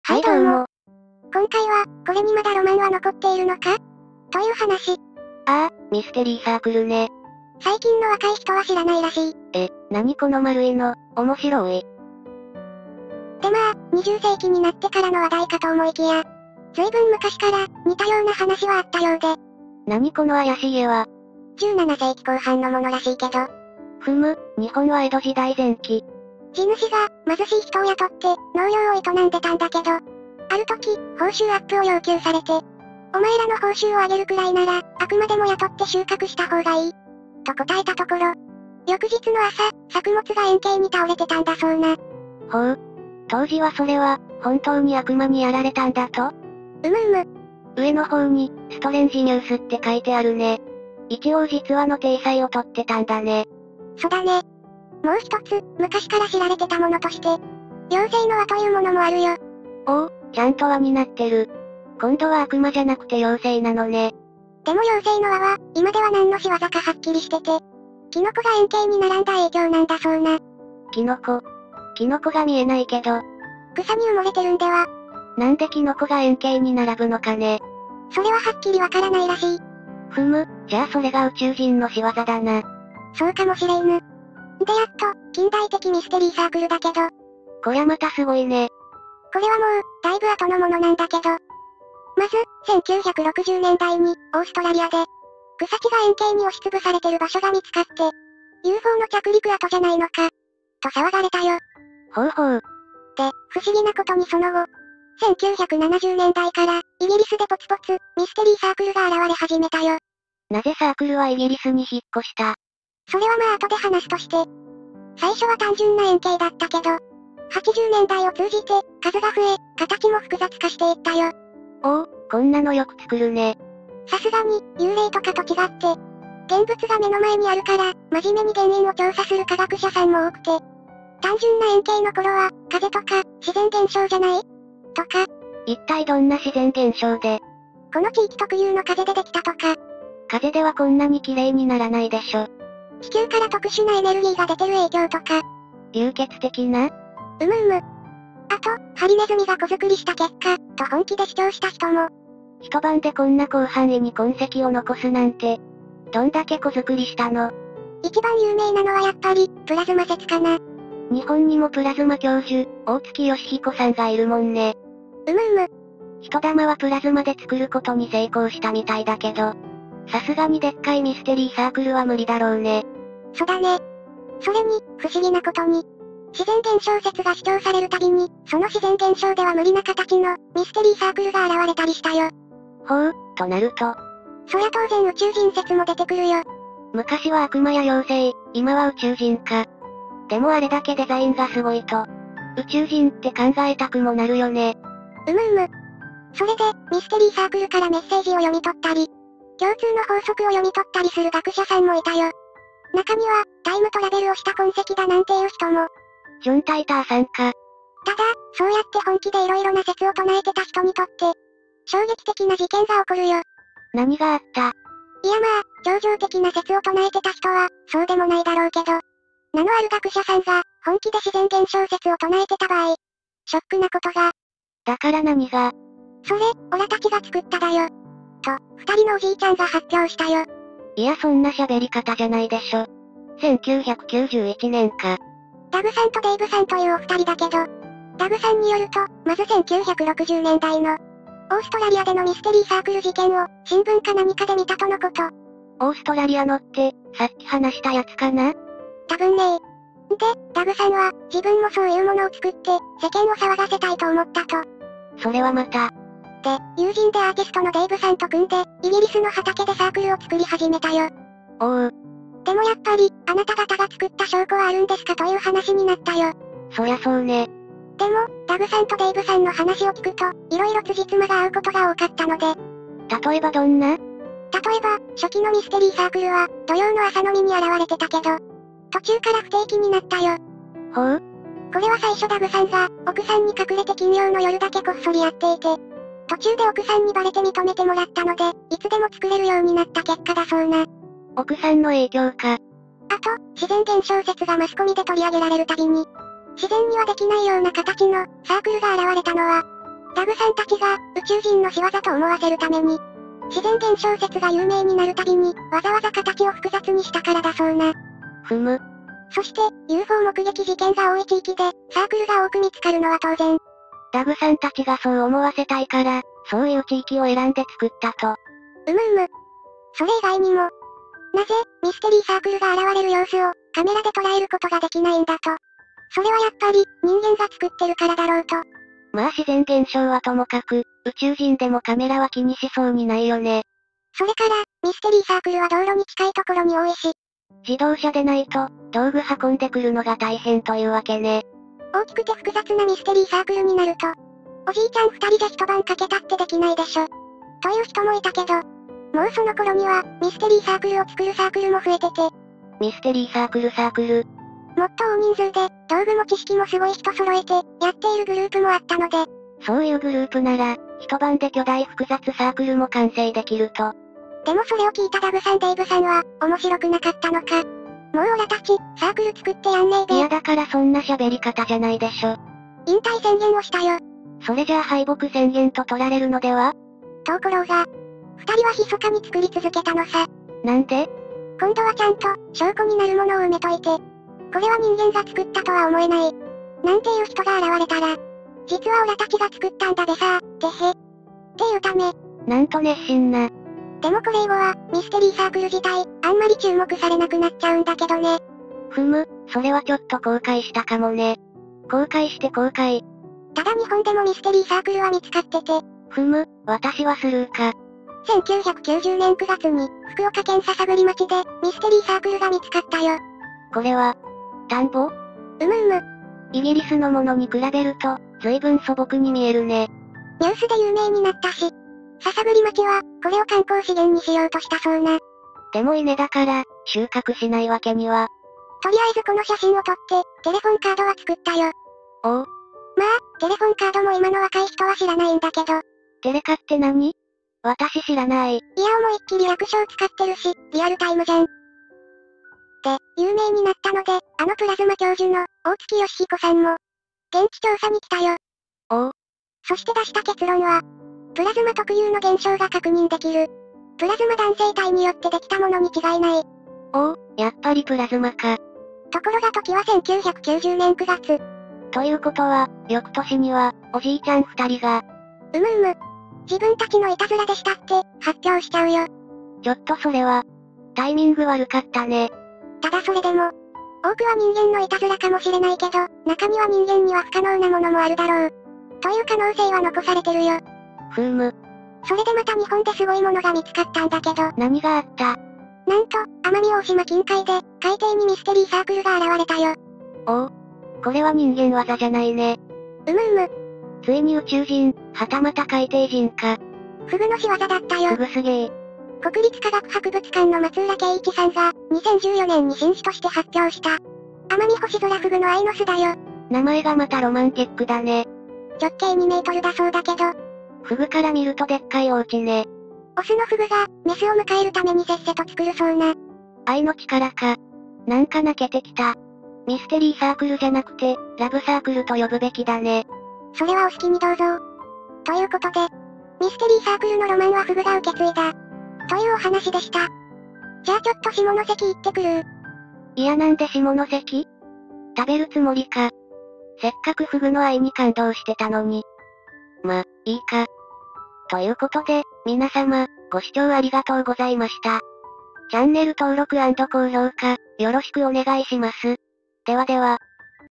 ミステリーサークルの正体～イギリスから日本へ、作ったのはUFO?宇宙人？ではなく〇〇【ゆっくり解説不思議】
当チャンネル全動画目次